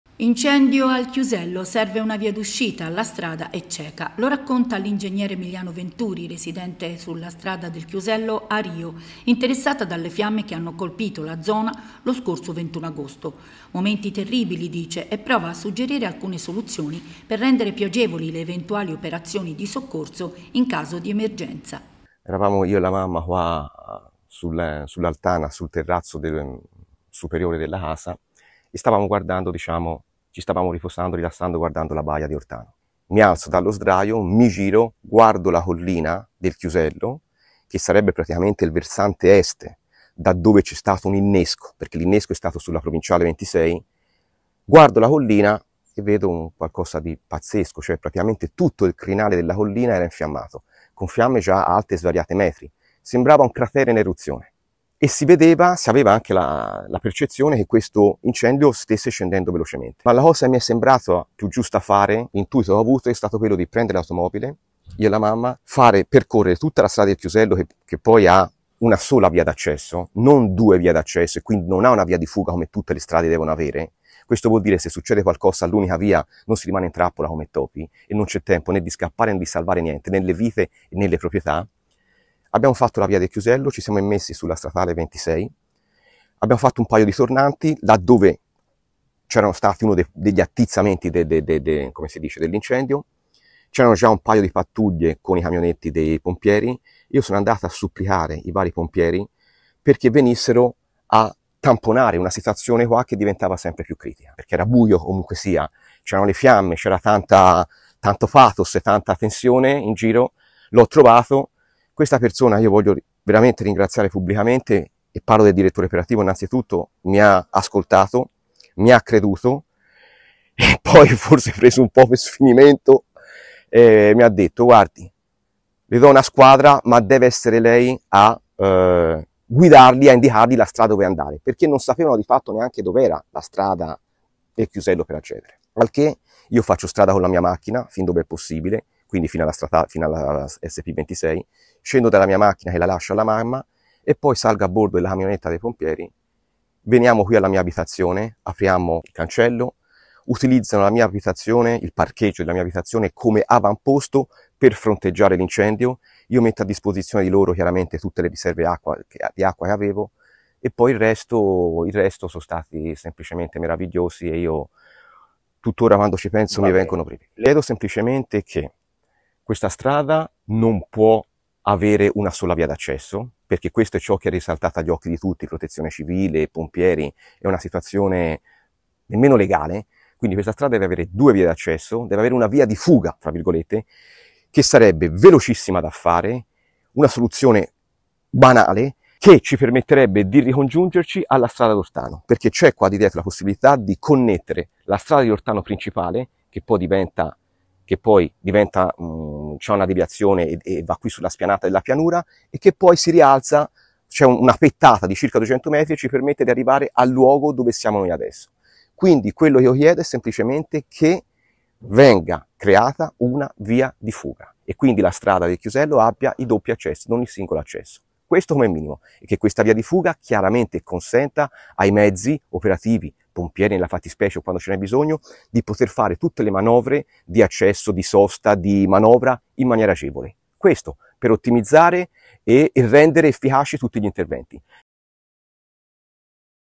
Incendio, al Chiusello serve una via d'uscita, la strada è cieca
Solo un accesso, stretta e tortuosa, poco conosciuta. Tragedia sfiorata, serve attenzione